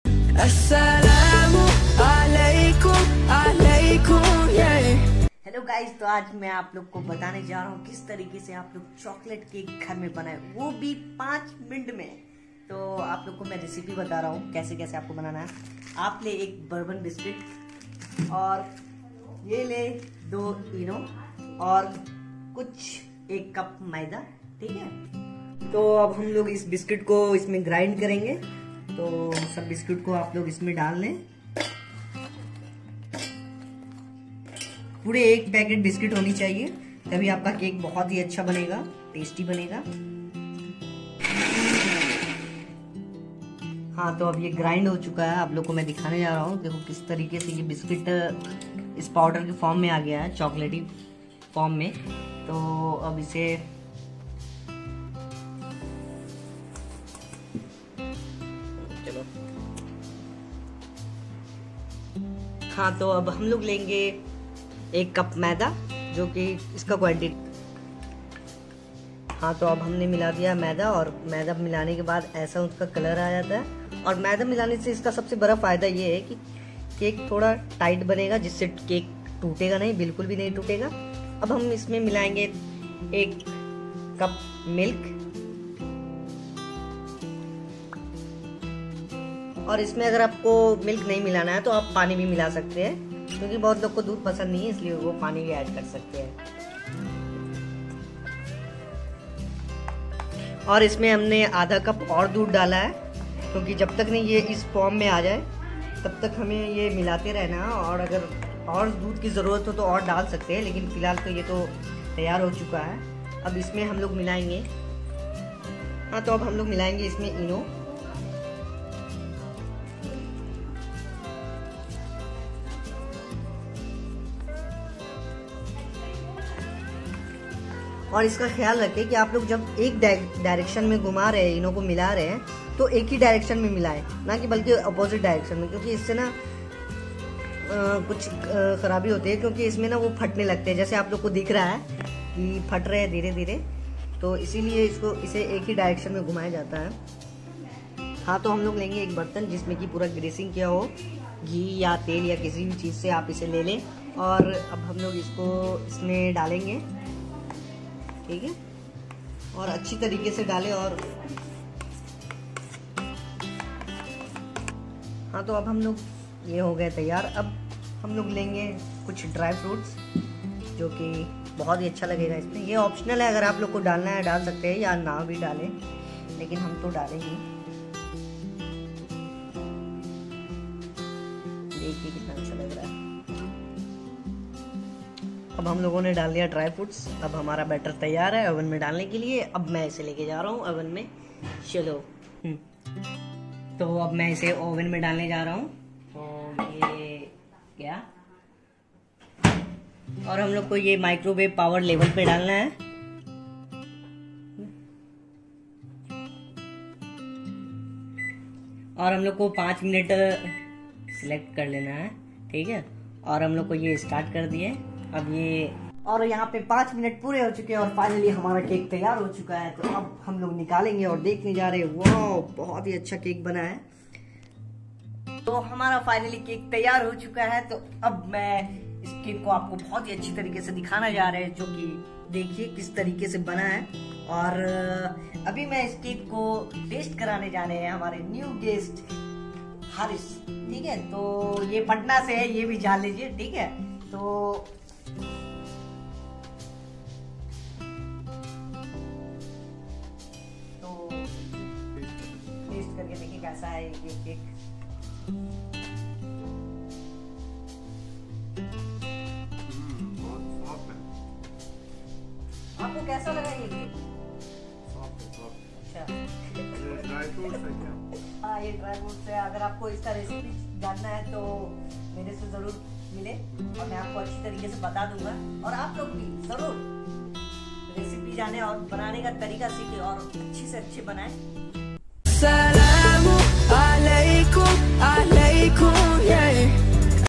How to make Biscuit cake in 5 minutes#cakein5minutes#biscuitcake
Ingredients :-

1. Bourbon biscuit - 1 pack
2. Maida (plain flour) - 1 cup
3. Milk or Water - 200 ml
4. ENO - 1 pack